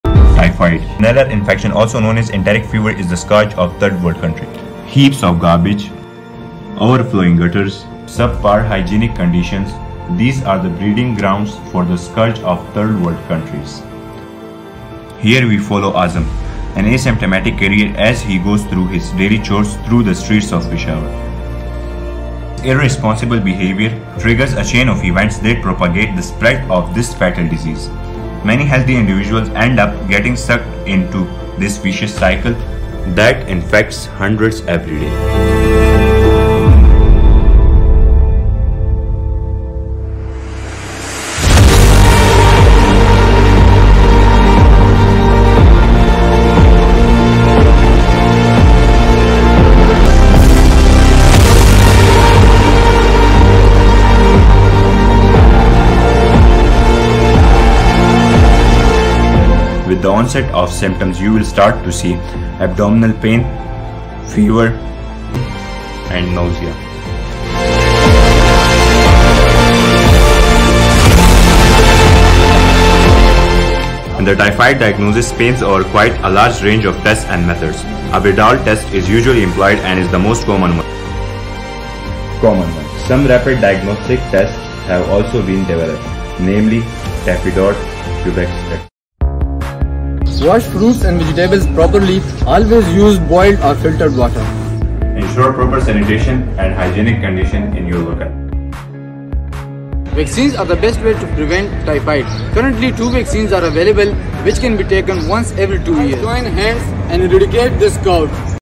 TYPHOID FEVER | a short documentary by medical students | KMC
Script and Direction : Abdullah Waheed
Team : Anara tania,  hasnain asad, sidra khan.
editor : Talha khan
videography : Haroon wazir & Muhammad Azam.
special thanks to : Haziq noor & ahmad Abdullah